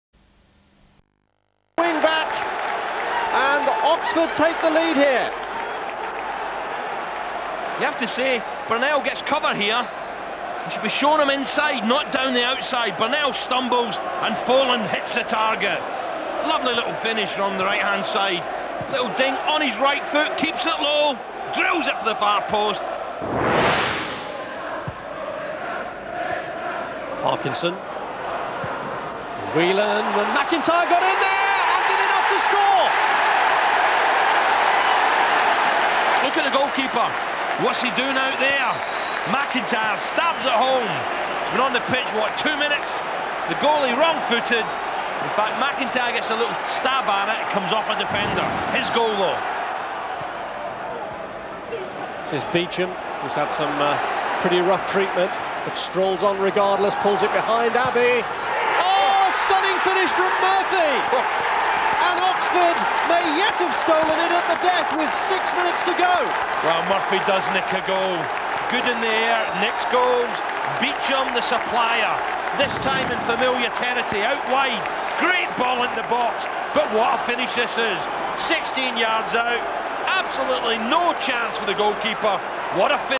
Reading 1 Oxford United 2 7th November 1999 Football League Division Two
A televised game, in which Rob Folland gives Oxford the lead.  Jim McIntyre levels, before Matt Murphy scores the winner.